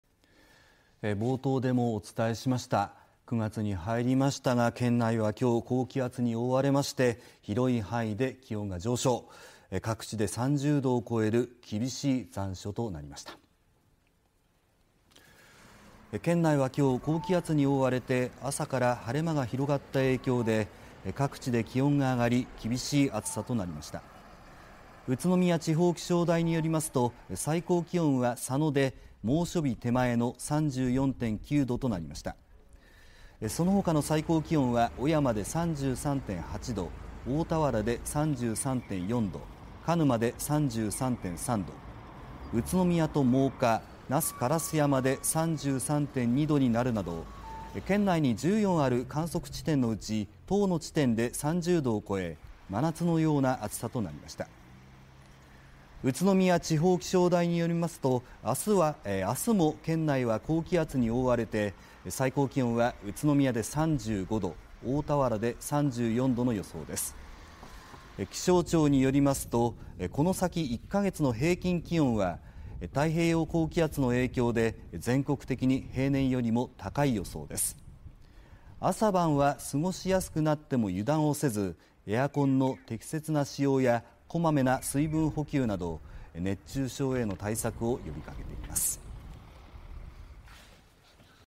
厳しい残暑 ６日の栃木県内１０地点で真夏日 (2024年9月7日)
９月に入りましたが栃木県内は６日、高気圧に覆われて広い範囲で気温が上昇し、各地で３０度を超える厳しい残暑となりました。

栃木県内は６日、高気圧に覆われ朝から晴れ間が広がった影響で各地で気温が上がり厳しい暑さとなりました。

宇都宮地方気象台によりますと、最高気温は佐野で猛暑日手前の３４・９度となりました。そのほかの最高気温は小山で３３・８度、大田原で３３・４度、鹿沼で３３・３度、宇都宮と真岡、那須烏山で３３・２度になるなど栃木県内に１４ある観測地点のうち１０の地点で３０度を超え真夏のような暑さとなりました。

宇都宮地方気象台によりますと７日も栃木県内は高気圧に覆われ最高気温は宇都宮で３５度大田原で３４度の予想です。

気象庁によりますとこの先１カ月の平均気温は太平洋高気圧の影響で全国的に平年よりも高い予想です。朝晩は過ごしやすくなっても油断せずエアコンの適切な使用やこまめな水分補給など熱中症への対策を呼びかけています。